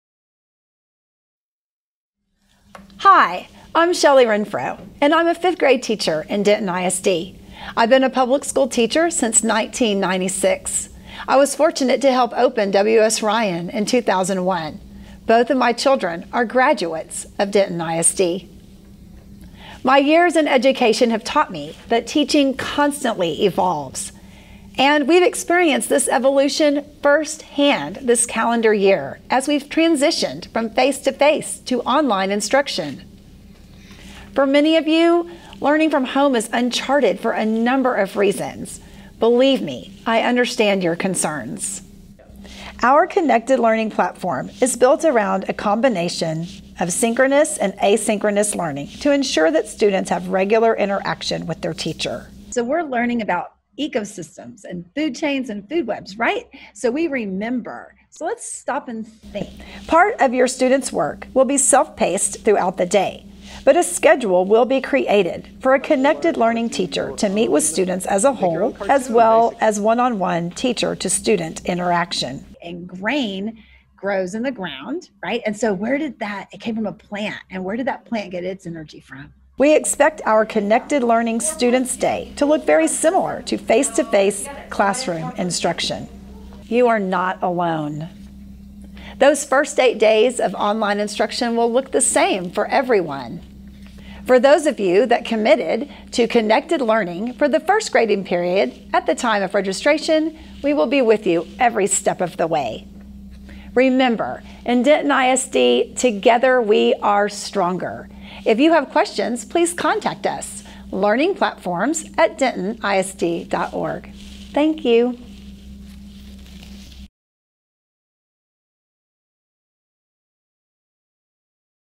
Elementary Online Learning
Former Denton ISD Teacher of Year, Shelly Renfro, takes us through the online learning process at the elementary level for the start of the school year.